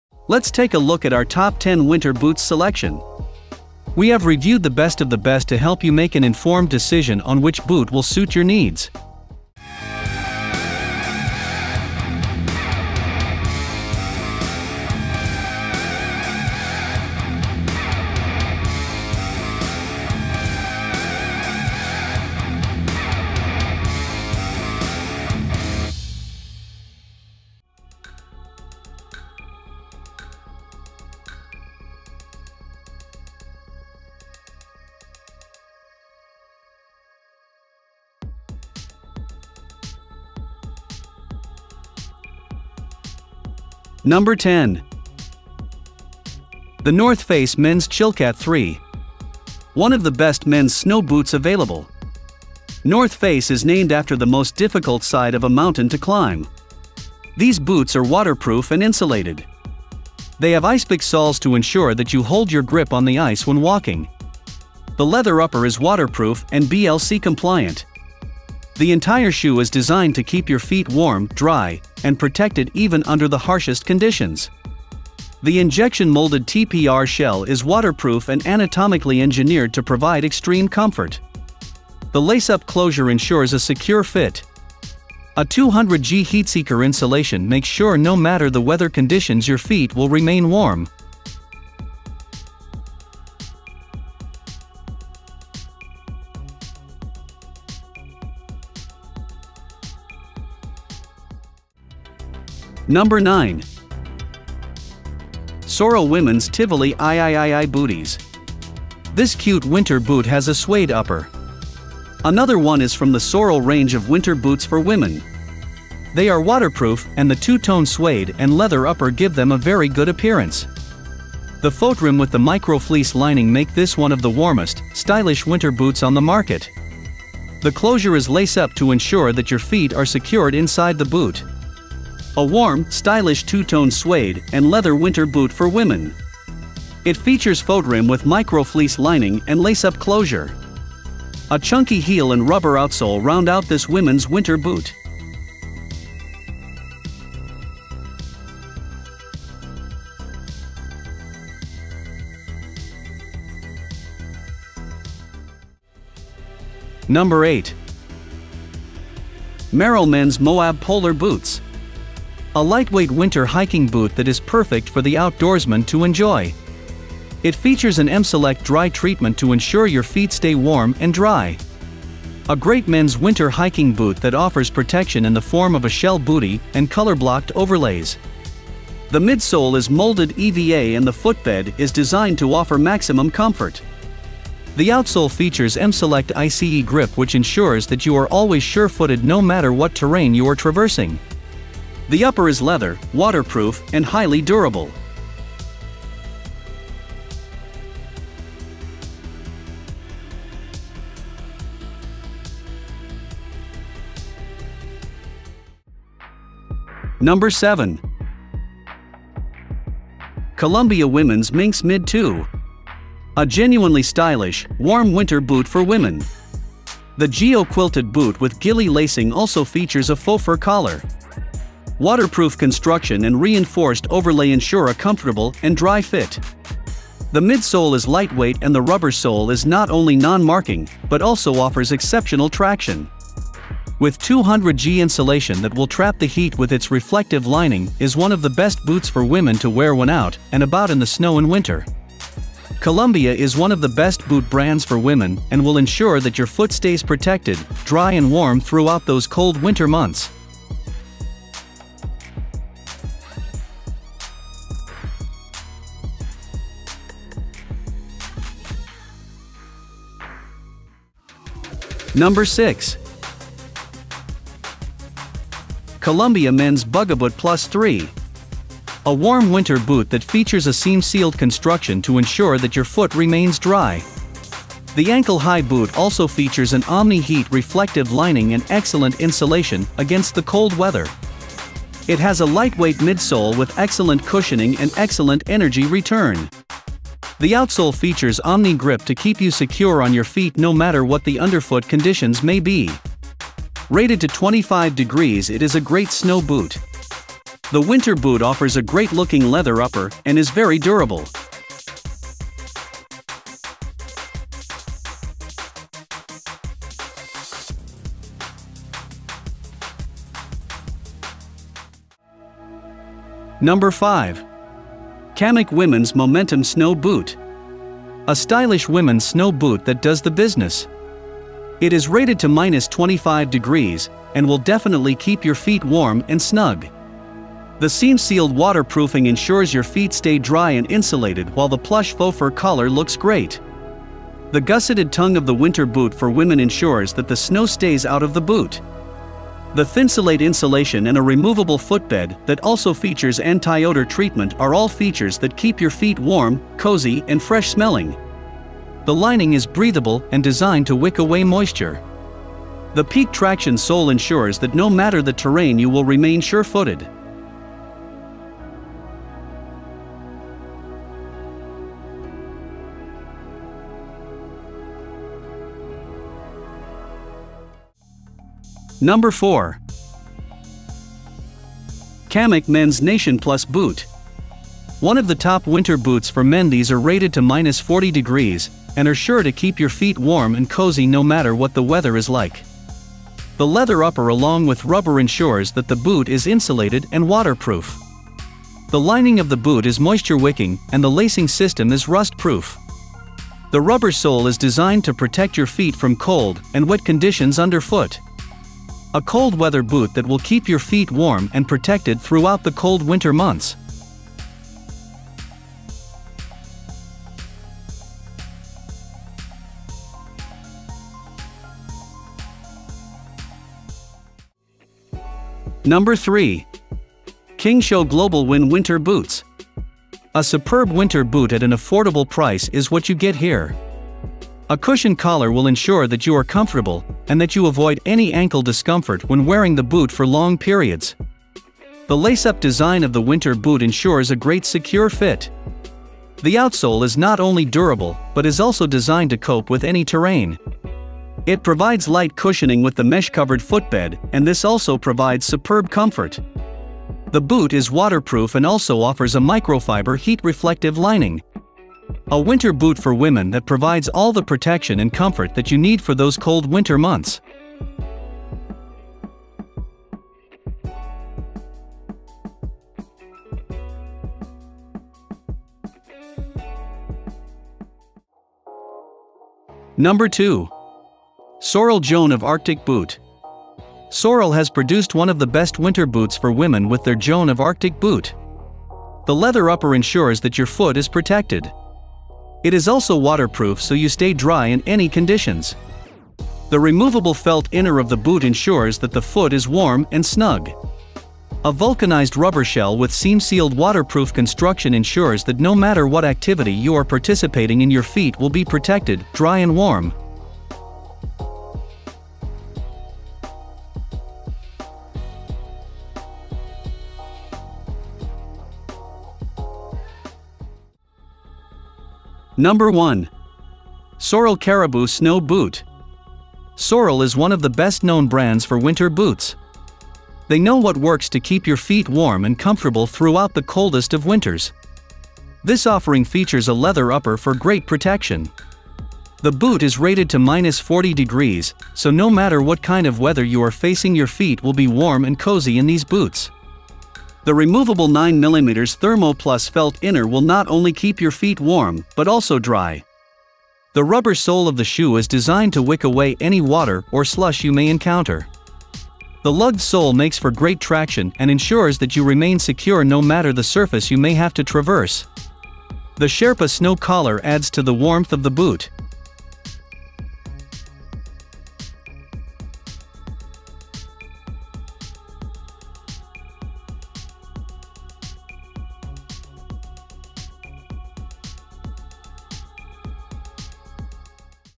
The 10 Best Winter Boots In 2022 - Survival Guide & Reviews!
Best Seller in 2022:
Our complete review, including our selection for the year's Best_Winter_Boots, BestShoesSeller is exclusively available on

These Best Winter Boots Video Buying Guide Included Products Are:

1.  Sorel Caribou Snow Boot

2. Sorel Joan Of Arctic Boot

3. Kingshow Globalwin Winter Boots

4. Kamik Men’s Nationplus Boot

5. Kamik Women’s Momentum Snow Boot

6. Columbia Men’s Bugaboot Plus III

7. Columbia Women’s Minx Mid II

8. Merrell Men’s Moab Polar Boots

9. Sorel Women’s Tivoli III Booties

10. The North Face Men’s Chilkat III

Summer is nearly a thing of the past and winter is on the horizon. Having the best winter boots that will protect your feet and keep them warm is the only option now. A great pair of winter boots are an essential part of your winter wardrobe. Whether it is just for getting to work before you change into your heels, or for being out and about with your family and friends, winter boots are the best way to keep your feet warm.

Selecting the pair that is best for you can seem a little daunting. Once you know what type of activities you will be using your boots for it becomes a little easier though. We have taken a long hard look at the options out there and selected the top ten winter boots on the market today.